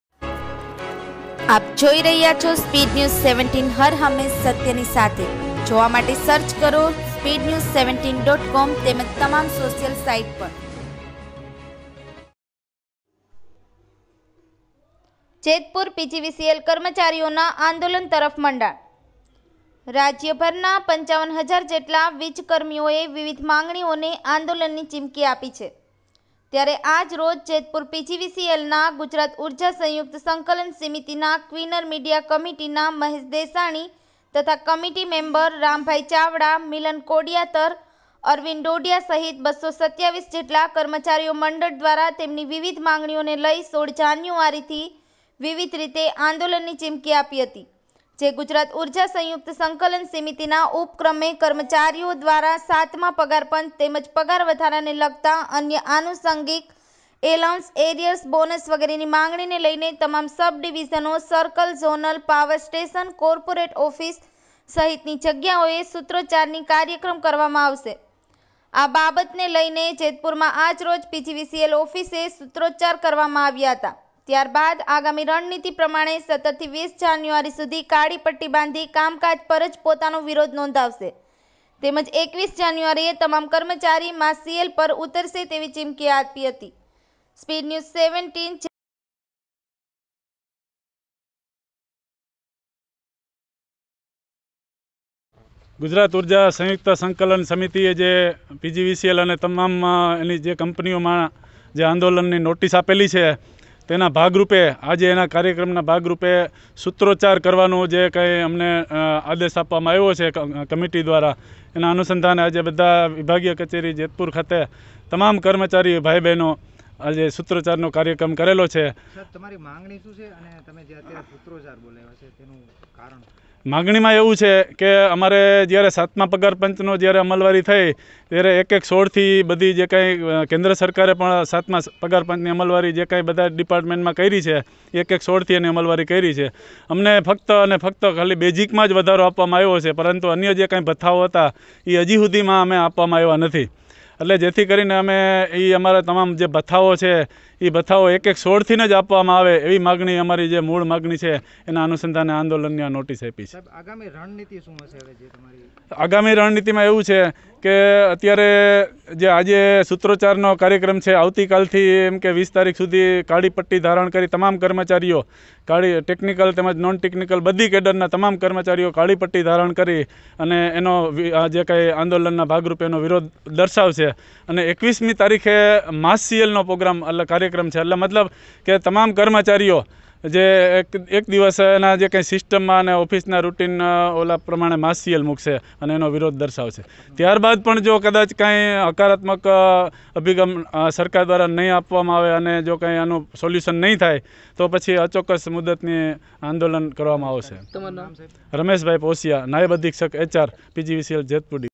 SPEEDNEWS17 જેતપુર પીજીવીસીએલ કર્મચારીઓના આંદોલન તરફ મંડાણ
જેતપુર પીજીવીસીએલ કર્મચારીઓના આંદોલન તરફ મંડાણ

રાજ્યભરના 55000 જેટલા વીજકર્મીઓએ વિવિધ માંગણીઓને આંદોલનની ચીમકી આપી છે.ત્યારે આજ રોજ જેતપુર પીજીવીસીએલનાં ગુજરાત ઉર્જા સયુંકત સંકલન સમિતિ ના કન્વીનર મિડિયા કમિટિનાં મહેશ દેશાણી, તથા કમિટિ મેમ્બર રામભાઈ ચાવડા, મિલન કોડીયાતર, અરવિંદ ડોડીયા સહીત 227 જેટલા કર્મચારીઓ મંડળ દ્વારા તેમની વિવિધ માંગણીઓને લઈ 16 જાન્યુઆરીથી વિવિધ રીતે આંદોલનની ચીમકી આપી હતી જે ગુજરાત ઉર્જા સંયુક્ત સંકલન સમિતિના ઉપક્રમે  કર્મચારીઓ દ્વારા સાતમા પગારપંચ તેમજ પગાર વધારા ને લગતા અન્ય આનુસંગિક એલાઉન્સ, એરિયર્સ, બોનસ વગેરેની માંગણીને લઇ ને   તમામ સબ ડિવિઝનો, સર્કલ ઝોનલ, પાવર સ્ટેશન, કોર્પરેટ ઓફીસ સહિતની જગ્યાઓએ સુત્રોચ્ચારની કાર્યક્રમ,કરવામાં આવશે આ બાબત ને લઇ ને  જેતપુર માં આજરોજ પીજીવીસીએલ ઓફીસે સુત્રોચ્ચાર કરવામાં આવ્યા હતા ત્યારે બાદ આગામી રણ નીતિ પ્રમાણે  17 થી 20 જાન્યુઆરી સુધી કાળી પટ્ટી બાંધી કામકાજ પર જ પોતાનો વિરોધ નોંધાવશે તેમજ 21 જાન્યુઆરીએ તમામ કર્મચારી માસ સી એલ પર ઉતરશે તેવી ચીમકી આપી હતી..

આ અગાઉ વીજ કર્મચારીઓ અને અધિકારીઓ દ્વારા આ માંગણીઓ કરવામાં આવી ત્યારે 3 મહિનામાં પ્રશ્ન નું નિરાકરણ થઇ જશે તેવી ખાત્રી ગુજરાત સરકાર ઉર્જા મંત્રી દ્વારા આપવામાં આવી હતી જે માત્ર લોલીપોપ હોવાનું કર્મચારીઓએ જણાવાયું હતું છતા કોઈ નિરાકરણ ના આવતા વિજ કર્મચારીઓ એ આંદોલનની ચીમકી આપી છે જેથી આવનારા સમયમાં સરકાર સામે મોરચો મંડાય તેવા એંધાણ દેખાઈ રહયા છે. કોરોના મહામારી, વરસાદ, ઠંડી ગરમી, વાવાઝોડા, પૂર સહિતની કુદરતી આફતોમાં પોતાના જીવ જોખમમાં મૂકી લોકોની સેવા કરતા વીજકર્મીઓની માંગણી પુરી થાય તેવું કર્મચારીઓ ઈચ્છી રહયા છે.